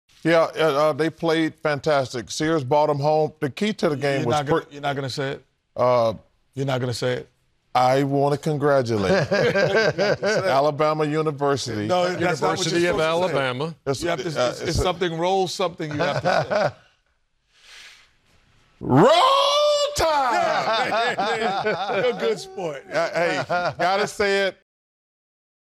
Alabama's first trip to the Final Four called for another first from Charles Barkley 🤣
Charles Barkley congratulates Alabama on their first trip to the Final Four.